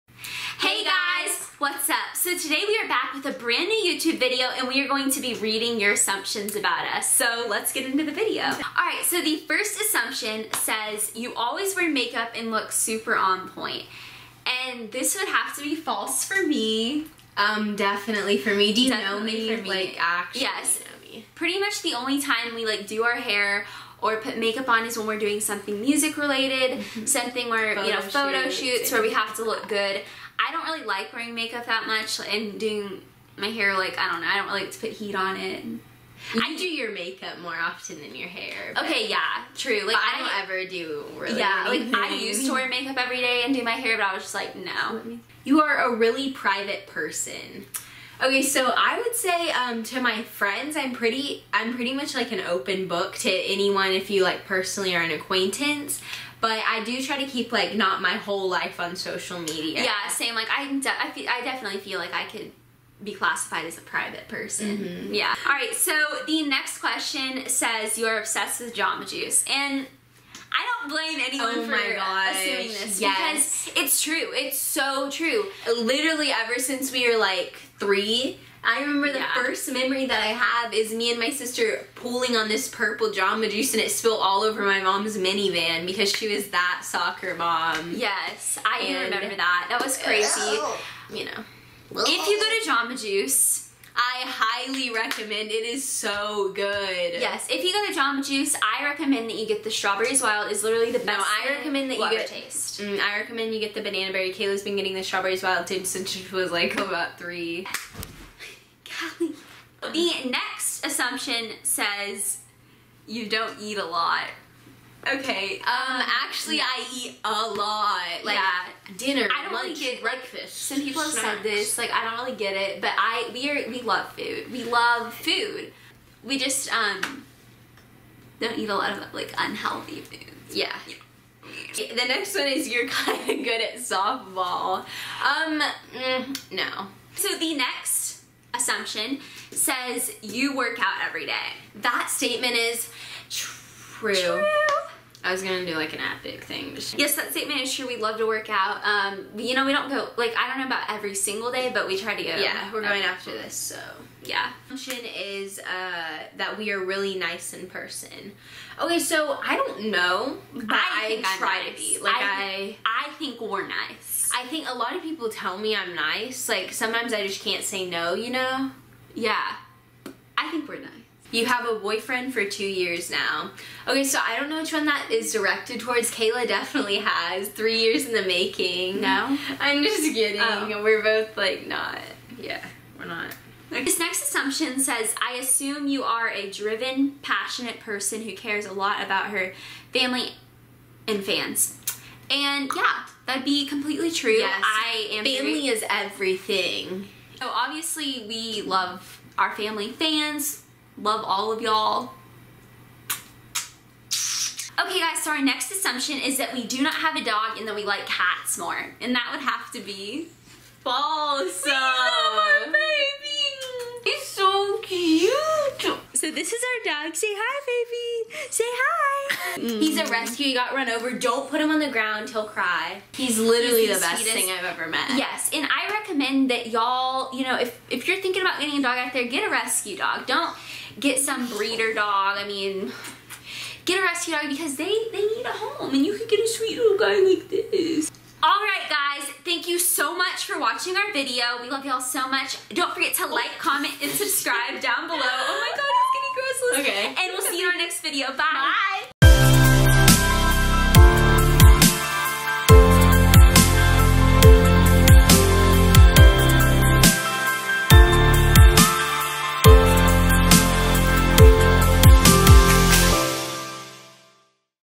READING YOUR ASSUMPTIONS ABOUT US....interesting | WILD FIRE
Thanks to everyone who participated in our assumptions video! Hopefully, we were able to answer many of your questions through these very common assumptions that are made. Love you guys!!!